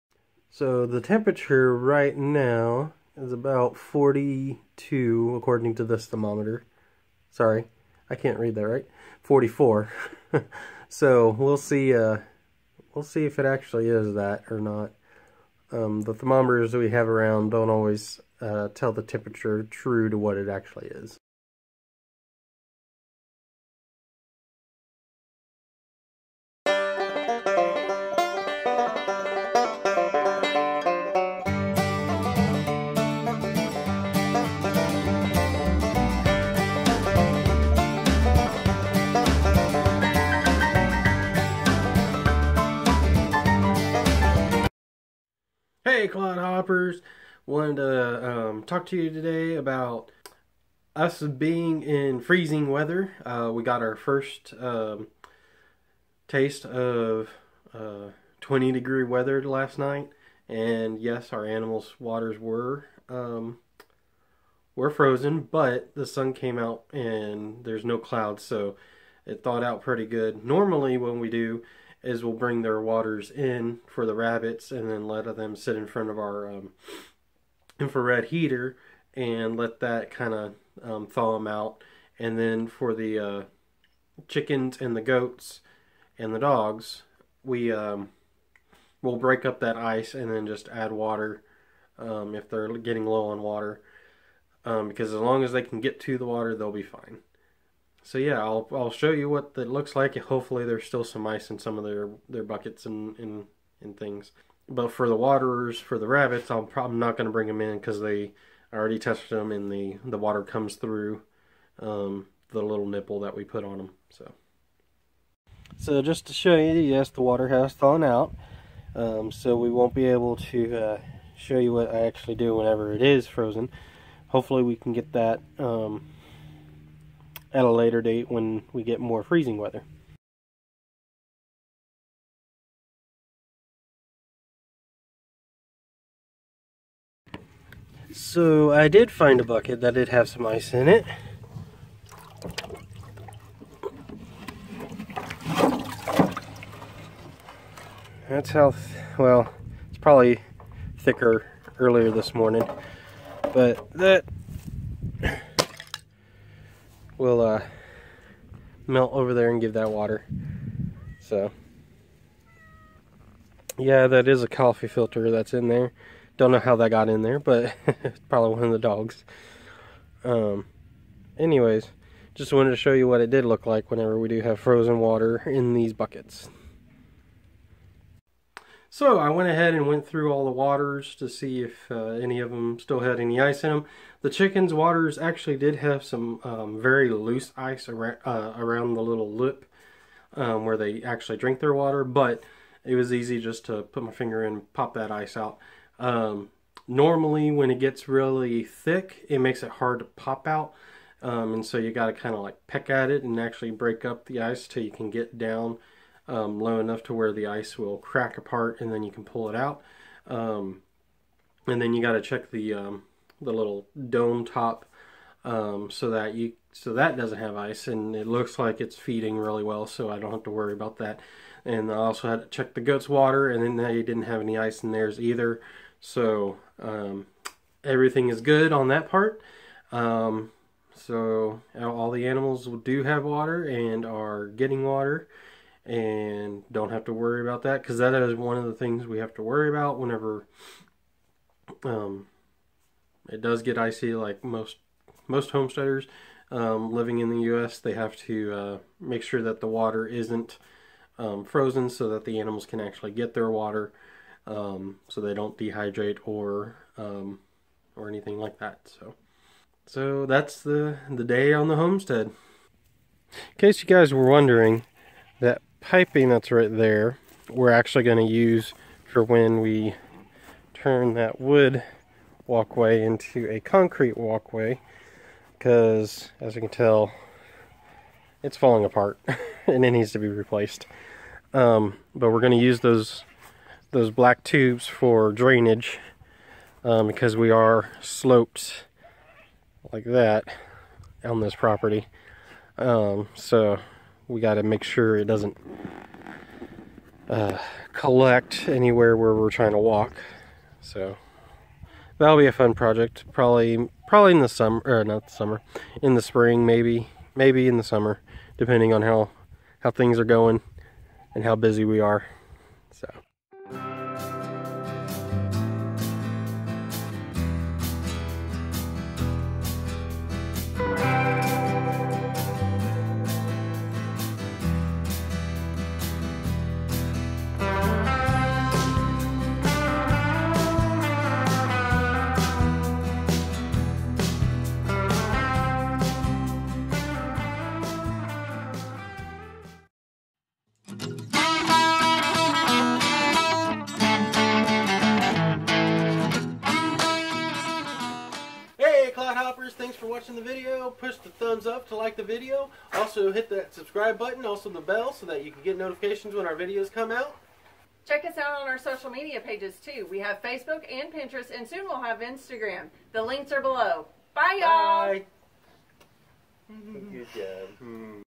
What? Freezing Weather in Texas?
Hot and humid Texas getting freezing weather? Yeah, it happens multiple times every winter.  We don't have long periods into the 20s (Fahrenheit) very often, but our temps dip that low periodically, especially during the night.  Justin explains what we do to make sure our animals have access to water when icy conditions hit the homestead.